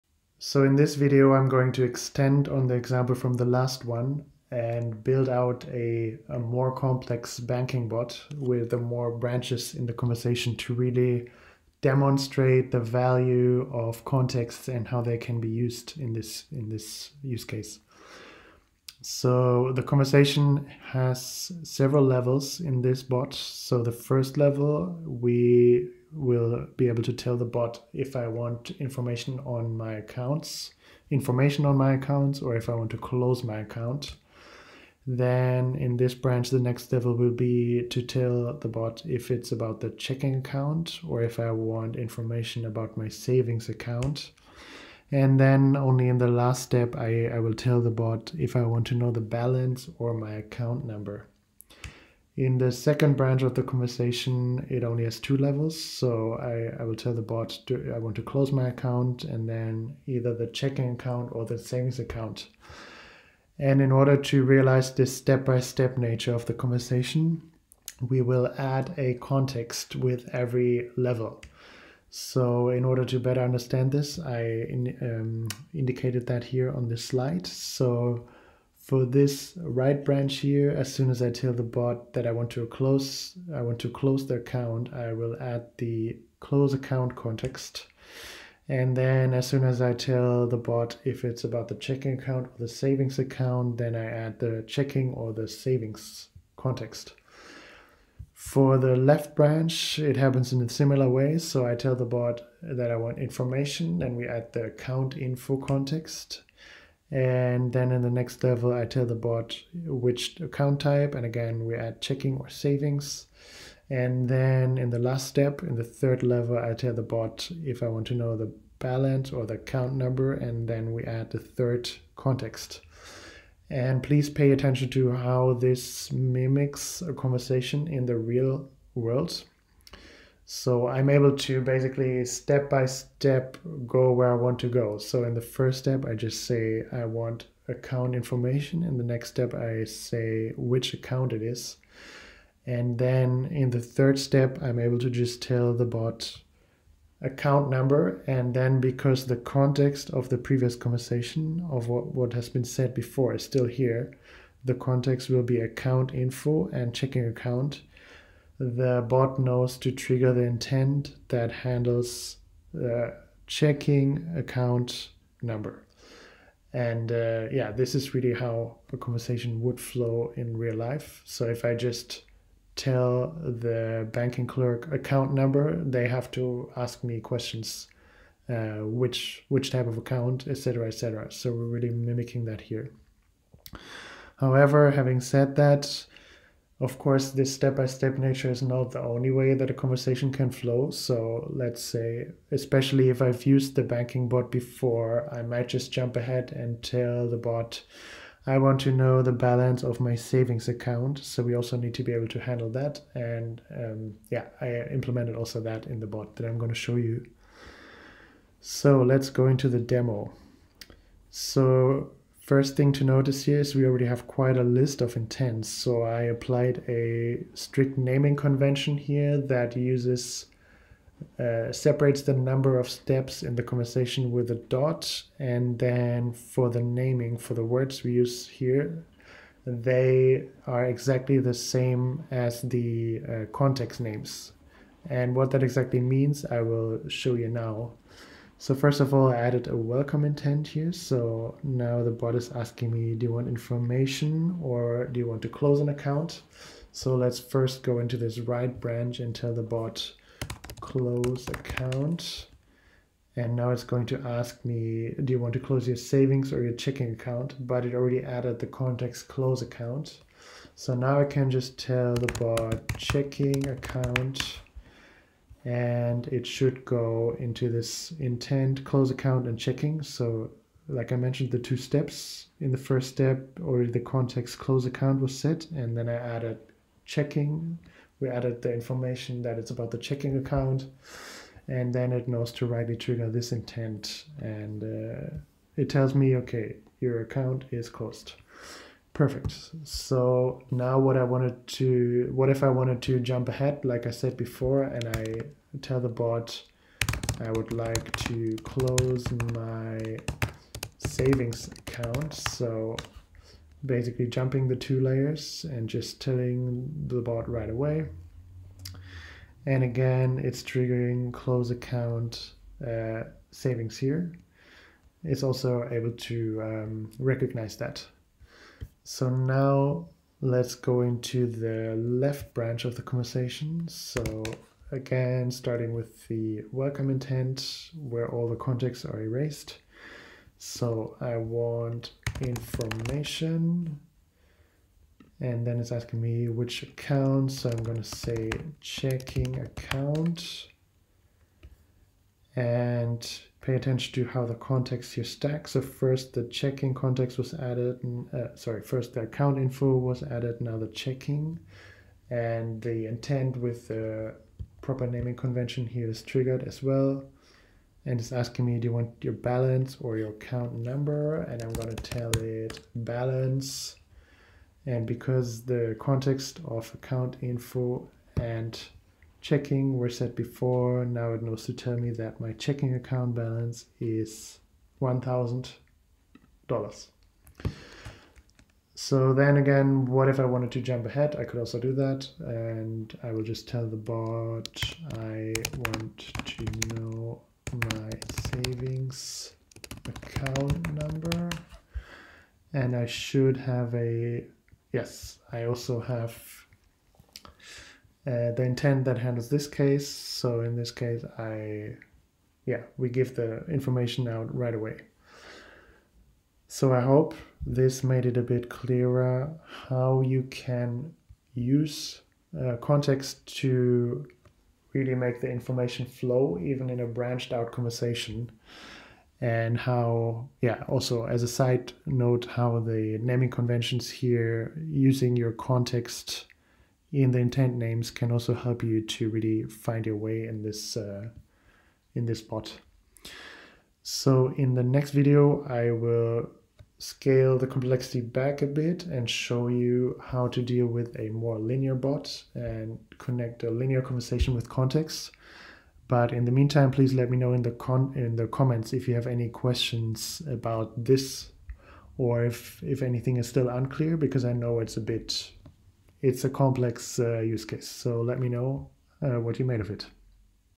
02 How to create a branched out conversation with context in Dialogflow ES
Fast-track your understanding of Dialogflow concepts and start developing your own advanced chatbot